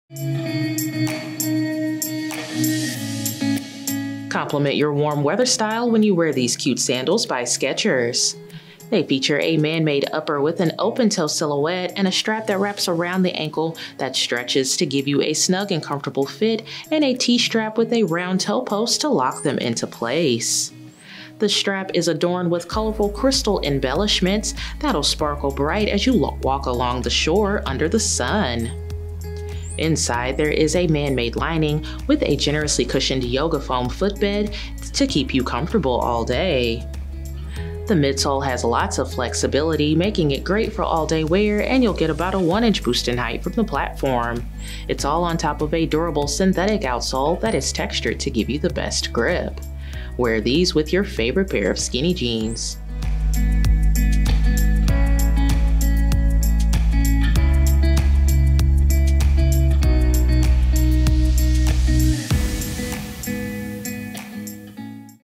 SKECHERS Meditation - Sweet Sparkle SKU: 9636906
Complement your warm weather style with a dash of color and sparkle featuring in the SKECHERS&reg; Meditation - Sweet Sparkle Sandals.
Textile lining.
Cushioned YOGA FOAM&reg; comfort footbed.
Slip-on style with elasticized slingback strap.
Rounded open toe.
Multicolored rhinestone embellishments on the upper.
Traction synthetic outsole.
Imported.